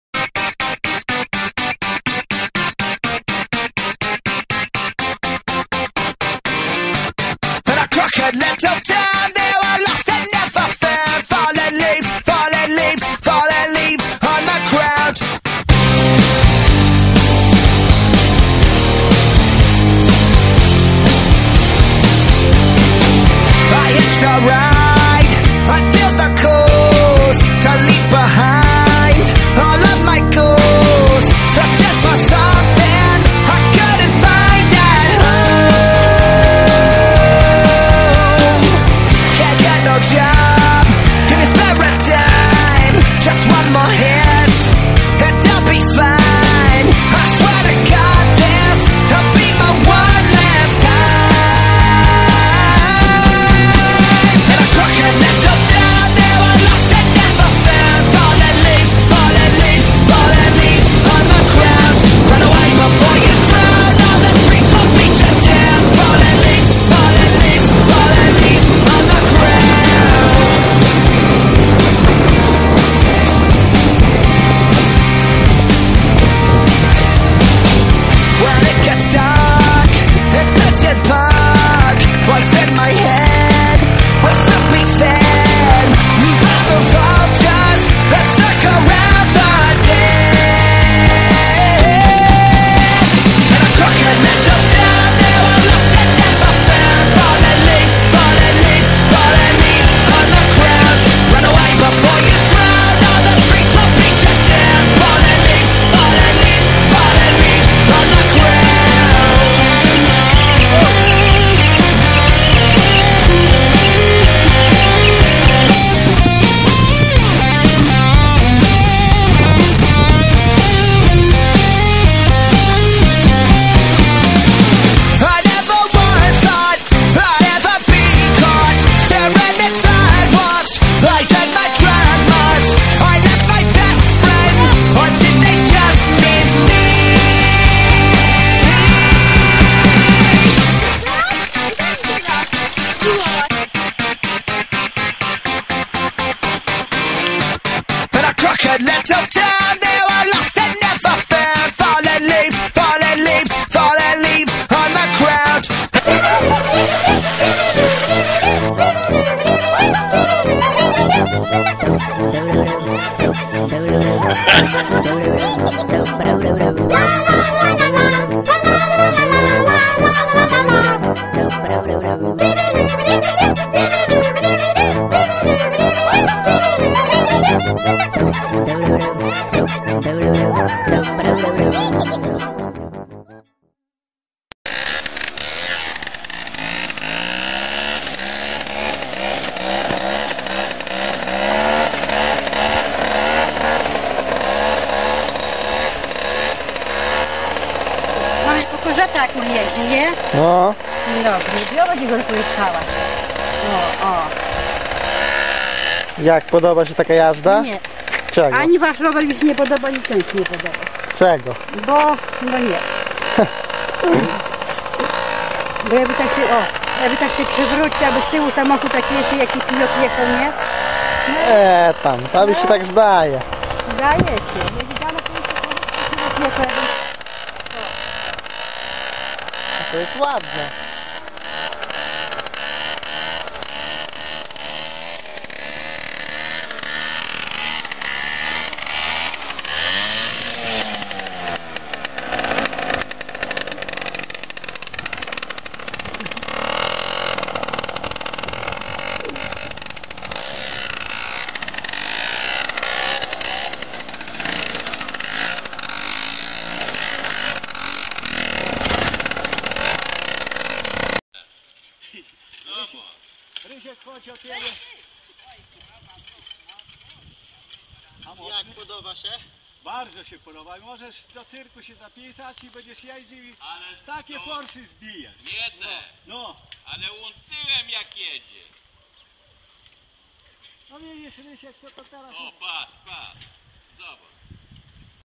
Moto Stunt Crew
Pierwszy film z serii Moto Stunt Crew. Podsumowanie sezonu 2008.